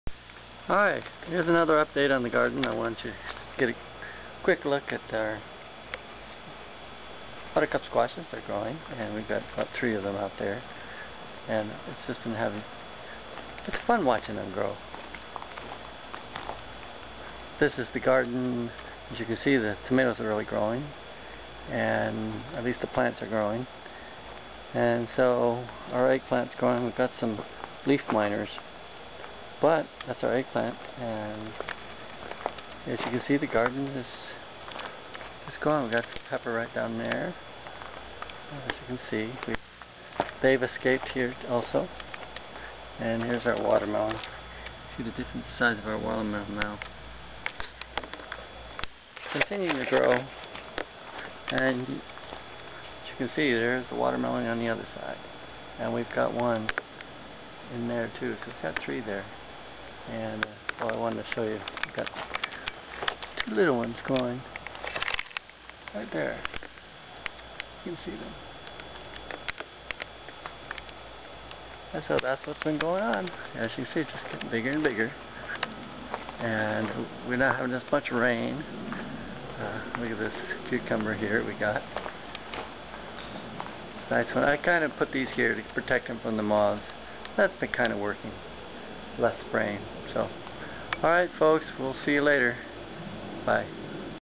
October Garden 2
Here's an update on the garden. Since making the video we have harvested watermelons, Amish snap peas, spaghetti and butternut squash, zucchini, garlic, and sweet potatoes. We just cooked and ate the best chicken noodle soup who's ingredients included our home grown vegetables.  We also ate two of our home grown sweet potatoes. Amazing taste !